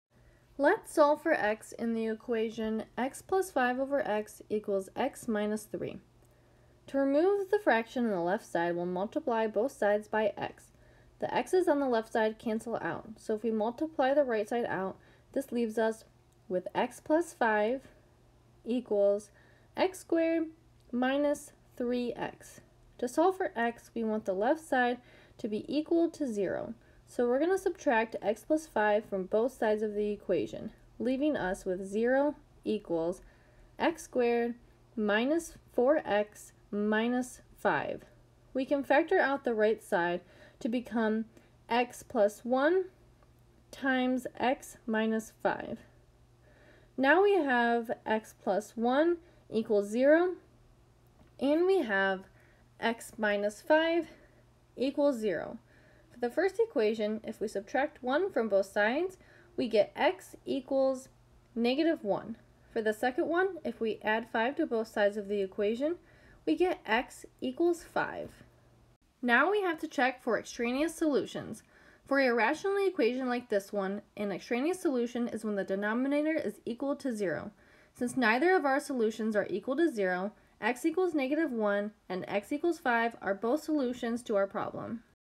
Rational Equation: Solve (x+5)/x = x-3
Erin from SVSU Micro Math helps you solve a rational equation.

Problem: Solve (x+5)/x = x-3